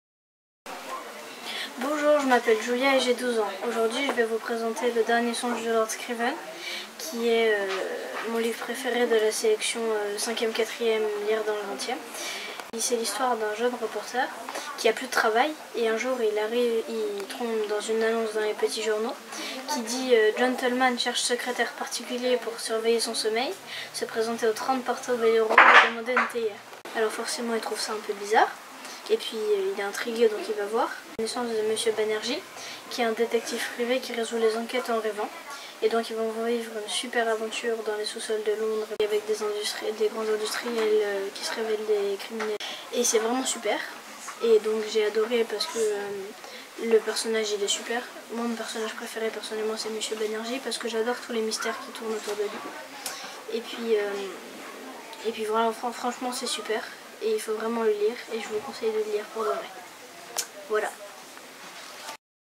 Giulia présente "Le Dernier songe de Lord Scriven"
Bravo à Giulia du collège Gambetta  pour sa première place au concours de critiques vidéo CritikBook dans le cadre du prix littéraire jeunesse Lire dans le 20e 2016/2017.